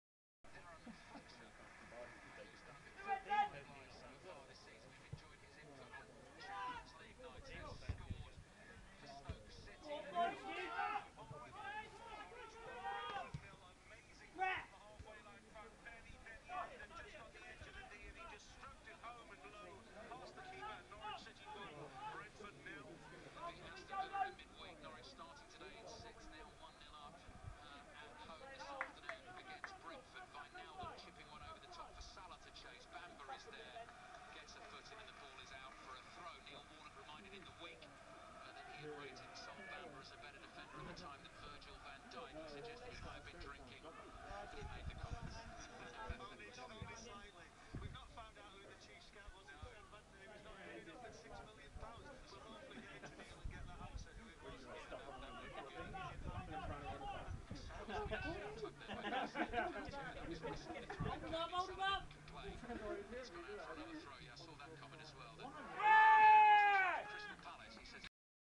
Duchy Div 3 Game, St Stephen Res 1 v 6 Liskeard Res
St Stephen defender Charlie Stone commits a foul in the area and a penalty is awarded to Liskeard n 48 minutes.